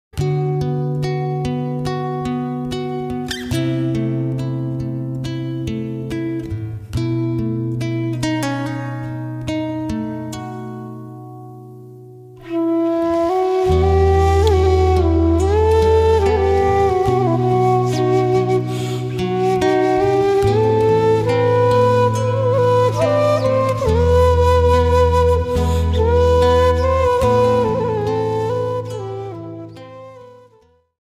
Detik-detik Pesawat jatuh 😭😭😭
Pesawat jatuh sayap patah

neng deso ningalan kedungtuban